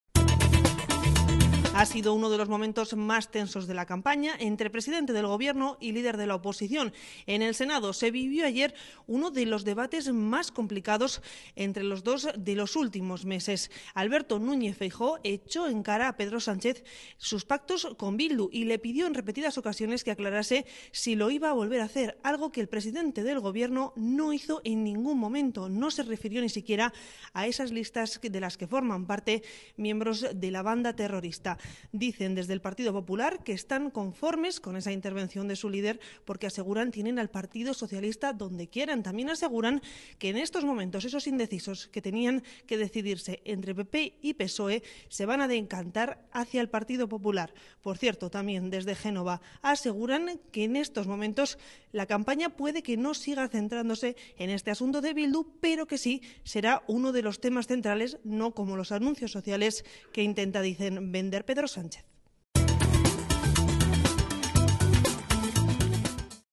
El PP "tiene a Sánchez donde quería" tras el escándalo de Bildu: "Le han reventado la campaña"
Destacan que el presidente del Gobierno no aclaró, por ejemplo, "si va a utilizar los votos de Bildu para gobernar en Navarra o en Pamplona".